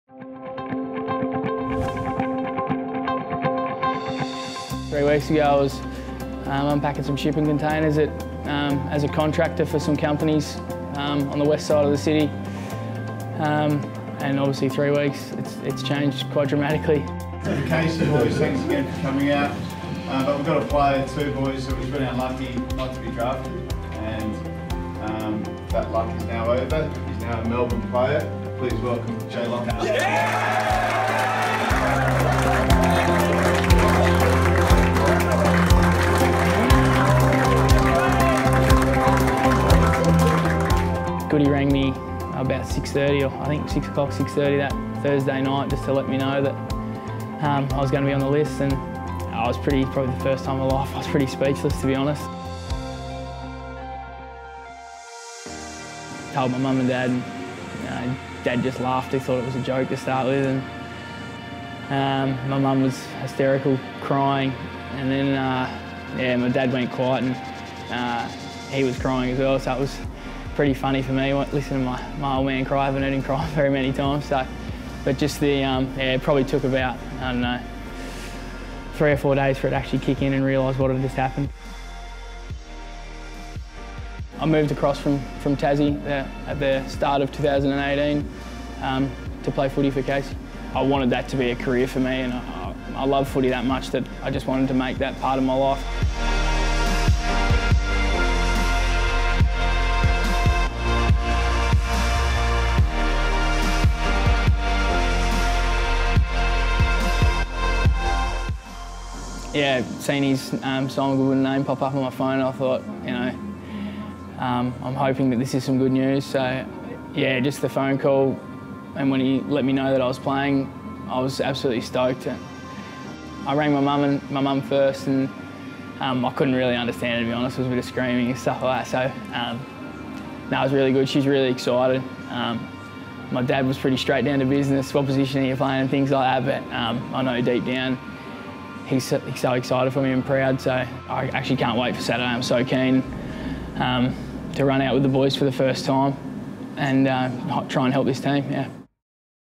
Debutant | Jay Lockhart
In just two weeks Jay Lockhart is elevated to the Melbourne list and selected for this weeks match against Geelong